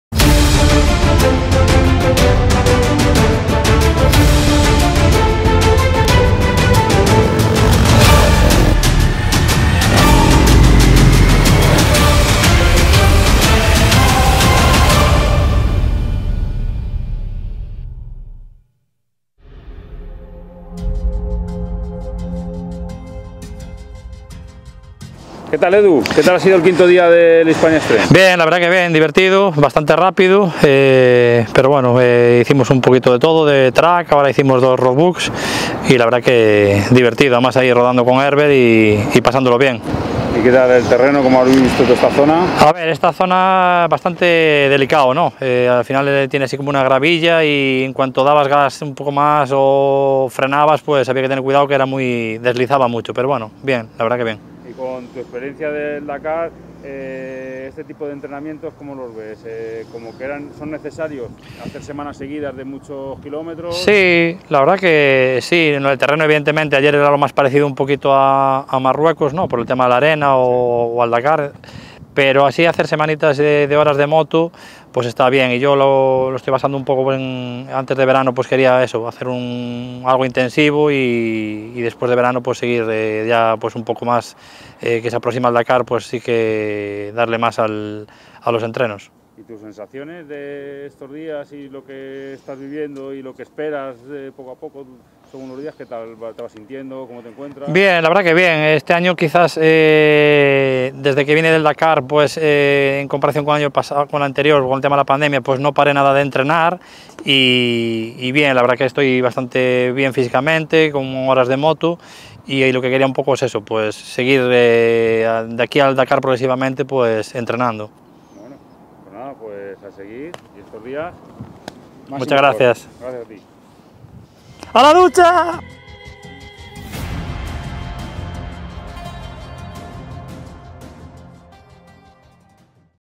Entrevista a Eduardo Iglesias piloto Monfortino en el
Eduardo Iglesias piloto gallego nos comenta su experiencia después de uno de nuestros entrenos.